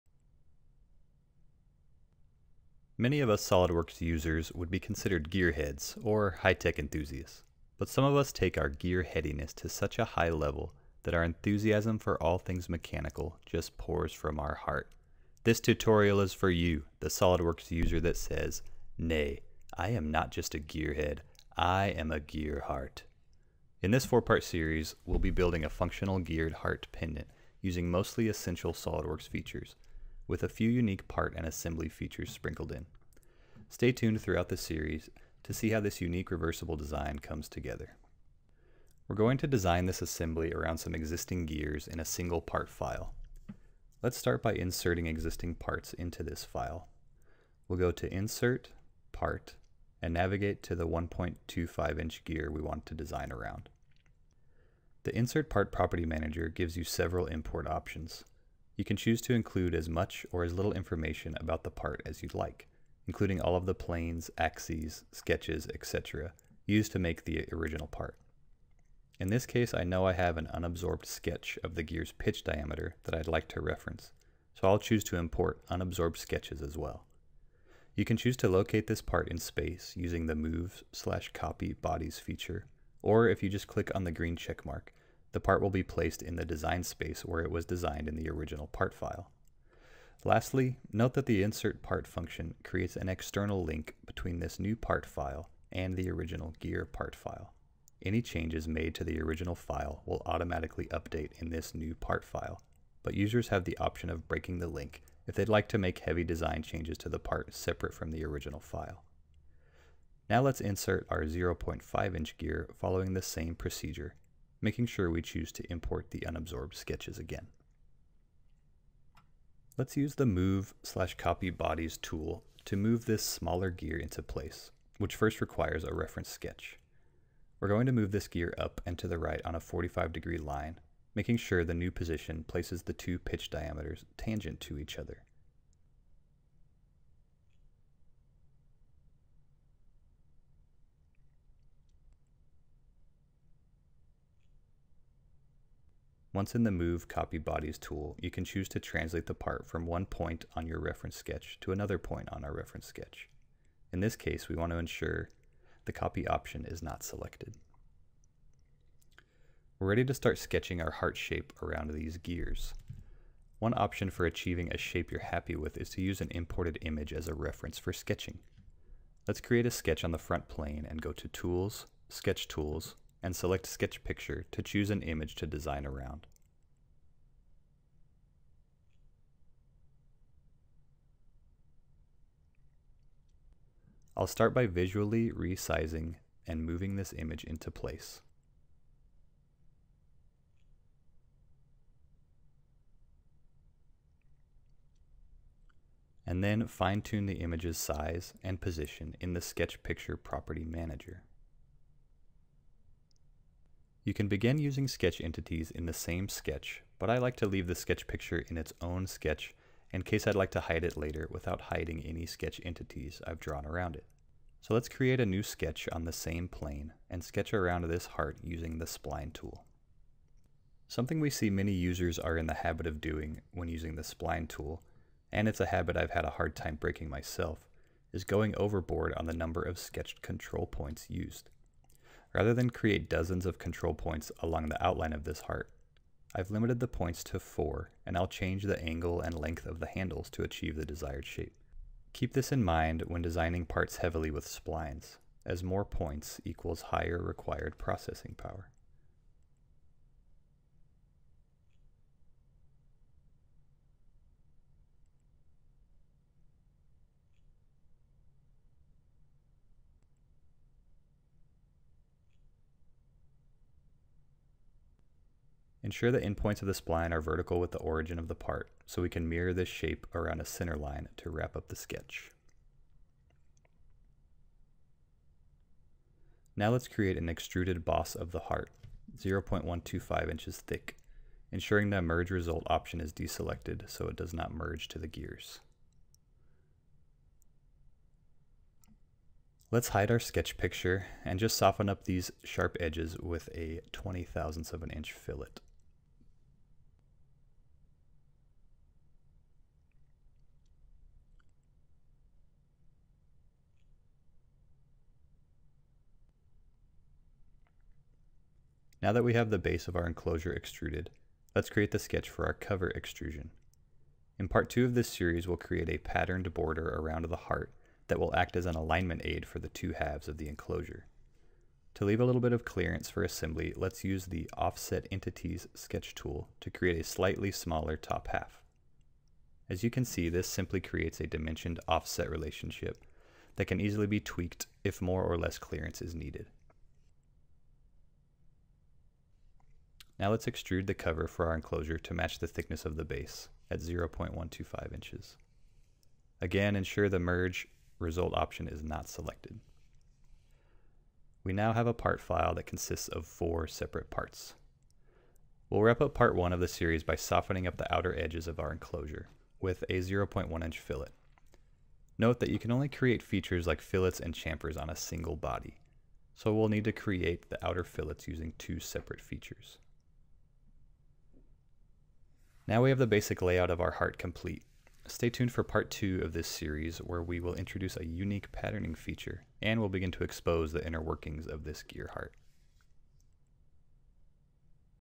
SOLIDWORKS Gear Heart Tutorial Series: Part 1
Many of us SOLIDWORKSusers would be considered gearheads, or high-tech enthusiasts.  But some of us take our “gear-headed-ness” to such a high level that our enthusiasm for all things mechanical just pours from our heart. This tutorial series is for you, the SOLIDWORKS user that says “Nay, I am not just a gearhead, I’m a gearheart.”  In this four-part series we’ll be building a functional geared heart pendant using mostly essential SOLIDWORKS features, with a few unique part and assembly features sprinkled in.

In part 1 of the series we will be designing around a few existing gears in a multi-body part file and introducing the sketch picture tool to assist in laying out the heart shaped enclosure.